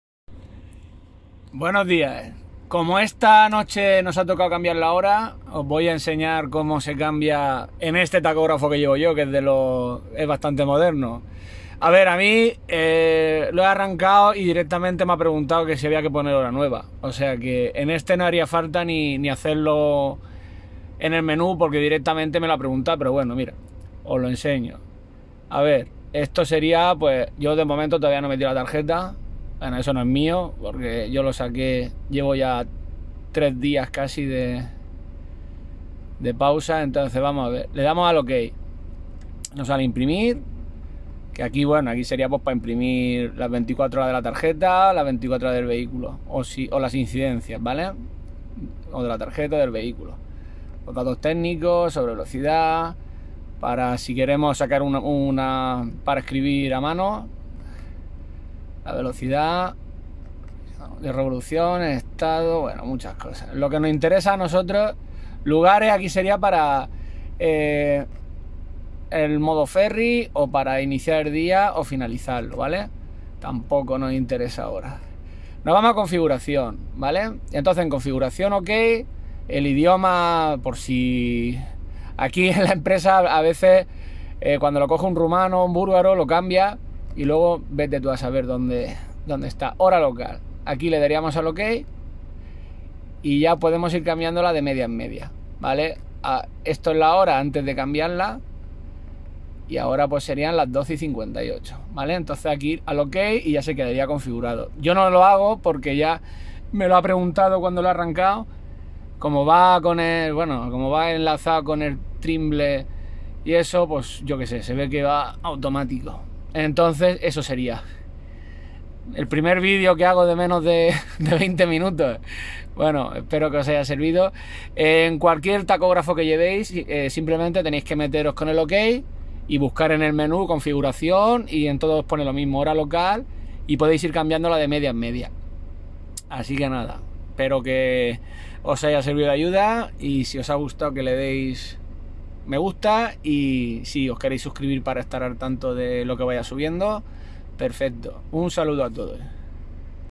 Cambiar la hora en el tacógrafo digital #23
Como hemos cambiado la hora esta noche pasada, os digo como cambiarla en el tacógrafo digital.